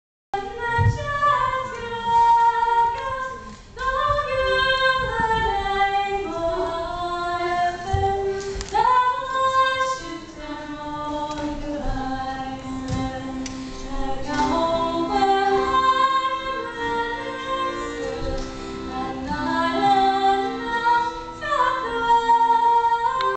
Joy singing
Joy Dunlop singing at a ceilidh - very short ( the clip, definitely not Joy teehee )